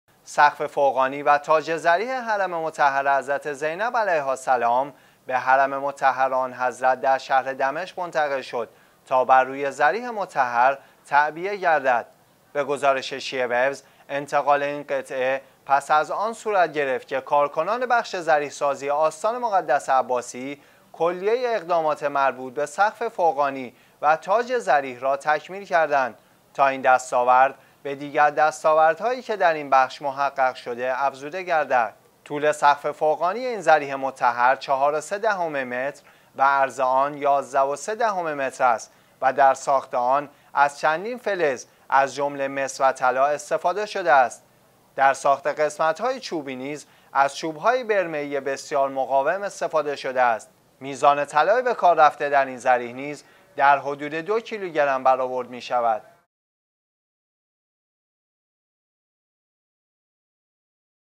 ورود تاج ضریح حرم مطهر حضرت زینب سلام الله علیها به دمشق
سقف فوقانی و تاج ضریح حرم مطهر حضرت زینب علیها السلام به حرم مطهر آن حضرت در شهر دمشق منتقل شد تا بر روی ضریح مطهر تعبیه گردد.
به گزارش شیعه ویوز، انتقال این قطعه پس از آن صورت گرفت كه كاركنان بخش ضریح سازی آستان مقدس عباسی كلیه اقدامات مربوط به سقف فوقانی و تاج ضریح را تکمیل کردند تا این دستاورد به دیگر دستاوردهایی كه در این بخش محقق شده افزوده گردد.
طول سقف فوقانی این ضریح مطهر 4.3 متر و عرض آن 11/3 متر است و در ساخت آن از چندین فلز از جمله مس و طلا استفاده شده است.
در ساخت قسمت های چوبی نیز از چوب های برمه ای بسیار مقاوم استفاده شده است.
 میزان طلای به کار رفته در این ضریح نیز، در حدود 2 کیلوگرم برآورد می شود.